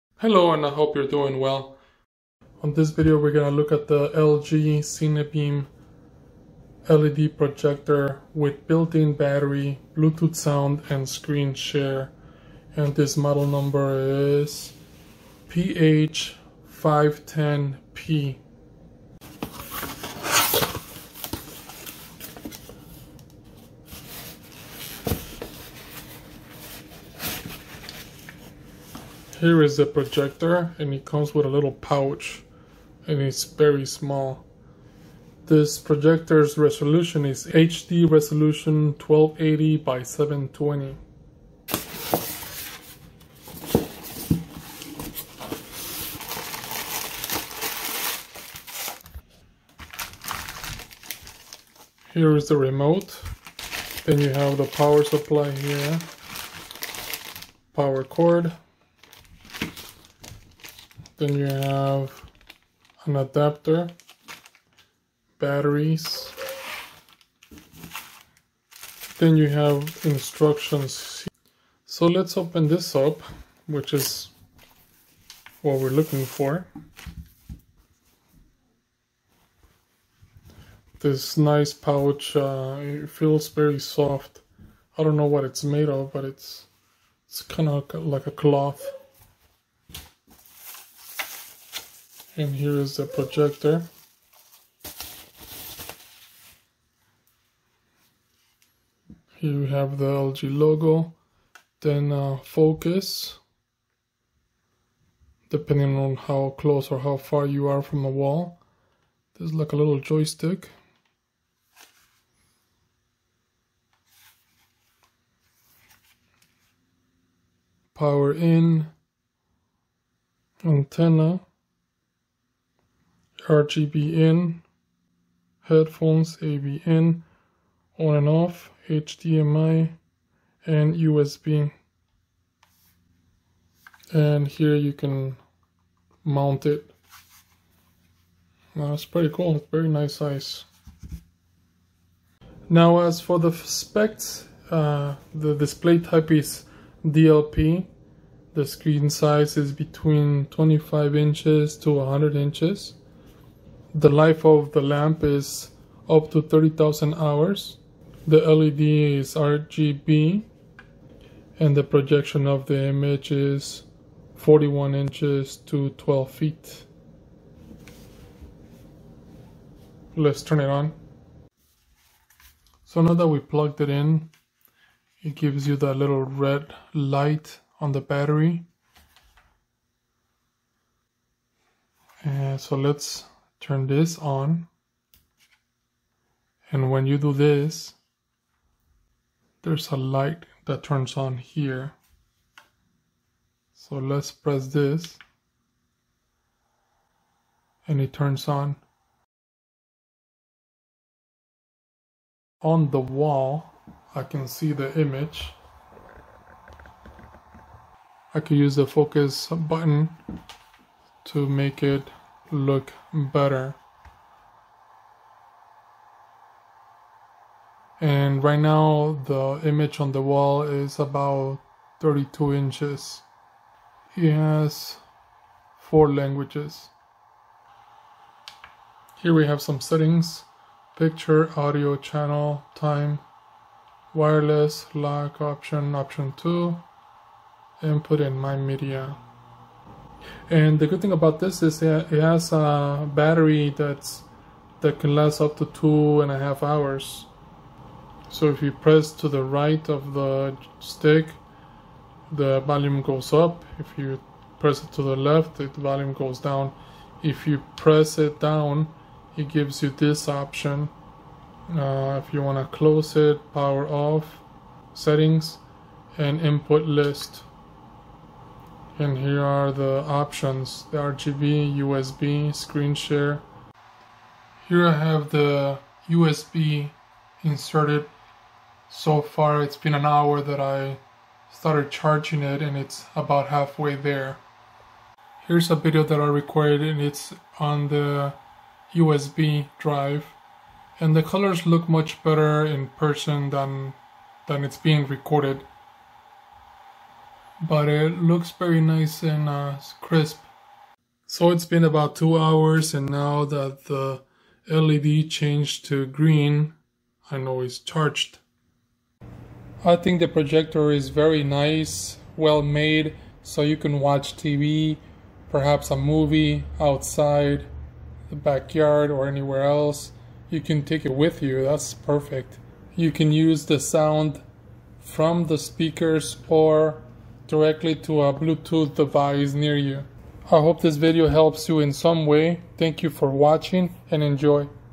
LG CineBeam Projector PH510P
The LGCineBeam LED Projector with Built in Battery, Bluetooth sound out and Screen Share is small but is awesome. It came with many things including instructions, remote, power cord and an adapter 3.5 to RCA. Everything was well packaged in the box and ready to use. It took about two hours to charge the battery and it lasts about two and a half hours, it is bright and can be mounted on a tripod or ceiling. The tuner is really good since the antenna I used was a simple one and picked up plenty of channels.

The projector has speakers and it’s not that loud but it can be connected fast to a Bluetooth speaker or a sound bar. It also has audio out for corded headphones.

The projector can mirror the cell phone; can be connected via HDMI, RGB or play from an USB.

The focus is manual and depending on the distance from the wall or screen, the image can be small or large.

The remote is very useful with input button, USB media, Channels, volume up and down,

The projector has a joystick that can access everything as well.

Overall the projector looks great; it is small but bright and very quiet.  The sound is not great but I did not expect it to be, it was easy to connect to a Bluetooth speaker, can be used with a screen or a wall.

You can find it at your favorite store and here are some links too:

LG PH510P

LG CineBeam PF510Q